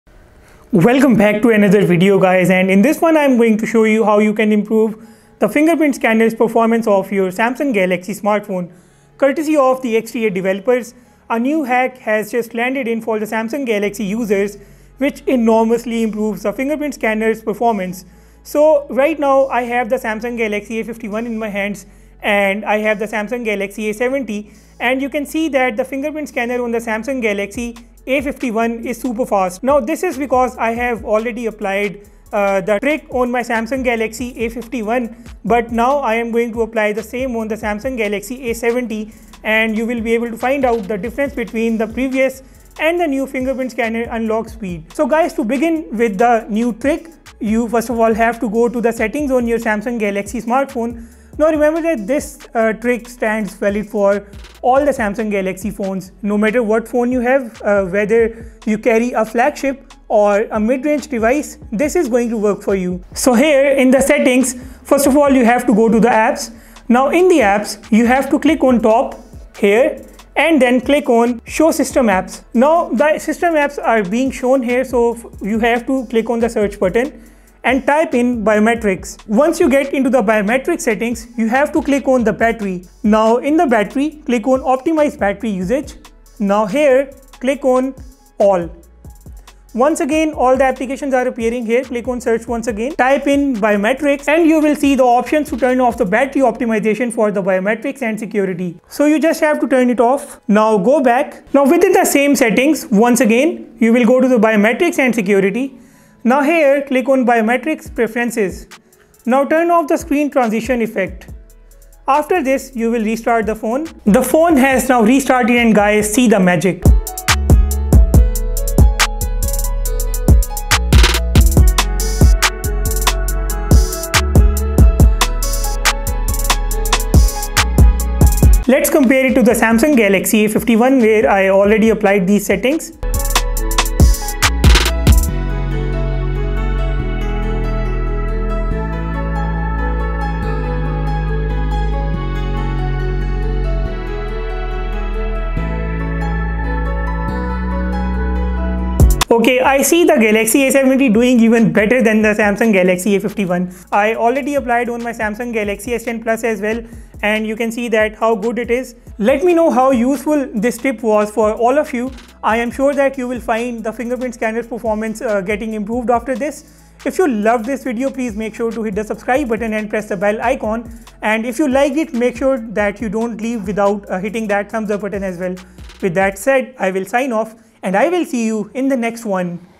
SAMSUNG FINGERPRINT SCANNER SPEED HACK - MAKE IT SUPER FAST
Hey Folks, a new trick has landed in order to improve the fingerprint scanner of the Samsung Galaxy smartphones. If you carry a Samsung phone with a fingerprint scanner, it's time to make it super fast with this all-new tip. This works on all Samsung phones with ultrasonic or the optical fingerprint reader. Watch this video carefully and improve Samsung fingerprint scanner speed right now.